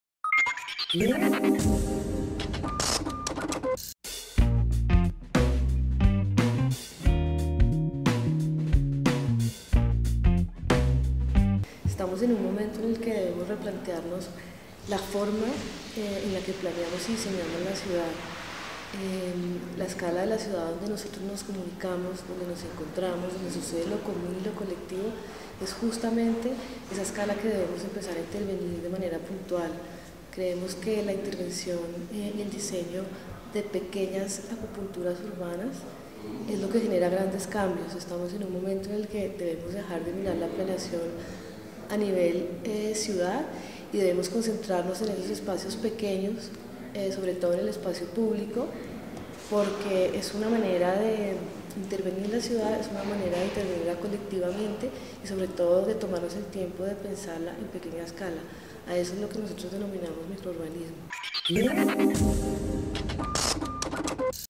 100ExpertosDicen Microurbanismo - Sharon Figueroa
Arquitecta con maestría en urbanismo. Trabaja en la Sociedad Colombiana de Arquitectos.